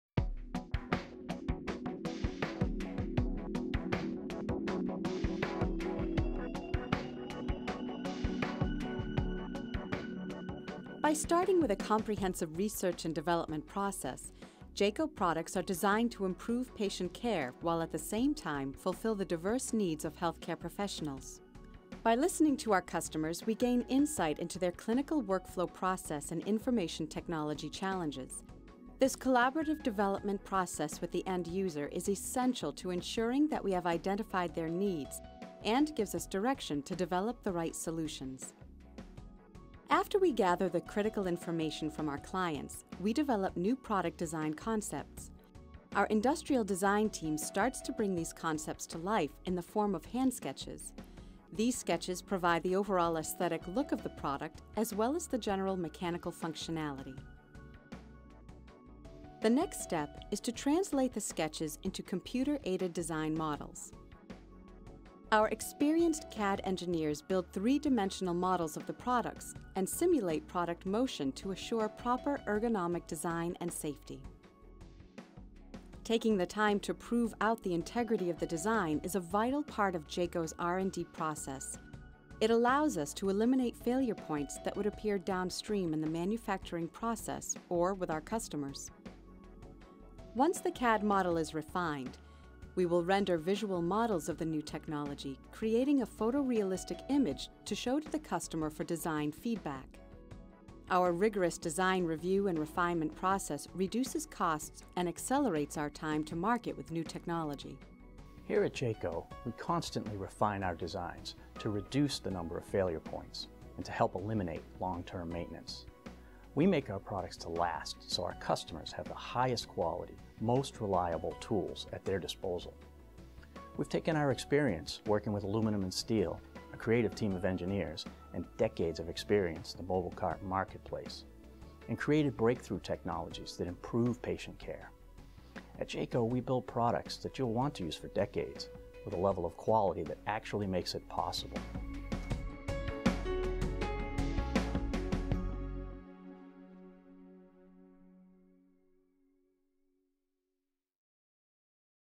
JACO Medical Cart Engineering: How It's Made
An overview of JACO, Inc's engineering process, outlining how Medical Computer Carts are designed and manufactured.